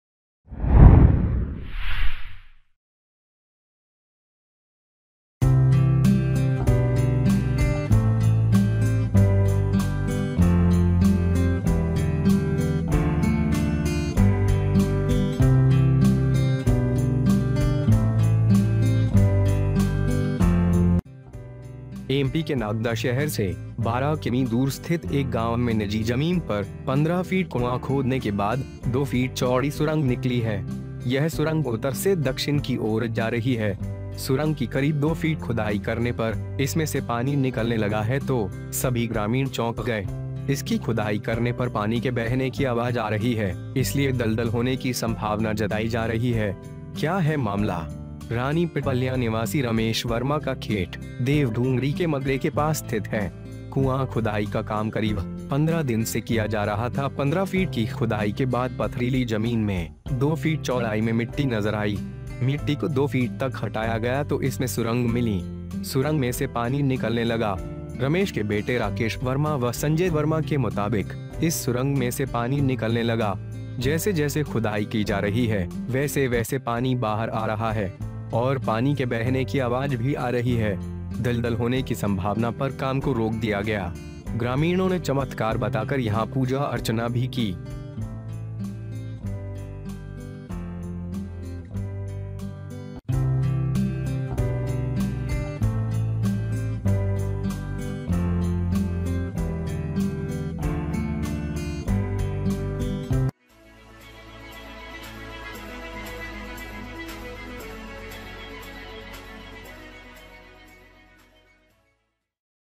कालिदास के 'मेघदूतम्' में है इस जगह का नाम, सामने आई रहस्यमयी सुरंग
In a village located 12 km away from Nagda town of MP, after a 15ft well was digged on a private ground, two feet wide tunnel came out. This tunnel is moving from north to south. Due to the excavation of about two feet of the tunnel, water has started coming out of it, all the villagers are shocked. There is a sound of water flowing when you dig it, so there is a possibility of swamps. What is the matter ...

Ramesh Verma's farm located in the heart of Goddess Dungari is located near the pulse of Lord Dungari.
- Excavation work was being done for nearly 15 days. After 15 feet excavation, soil appeared in two feet width in rocky ground.
If Mitti was removed for 2 feet then it got a tunnel.
Water started coming out of the tunnel
According to Ramesh's son Rakesh Verma and Sanjay Verma, water came out of this tunnel.
As water is coming out as well as the sound of water flowing as it is being excavated.
-The work was stopped on the possibility of movement The villagers also performed rituals here as a miracle.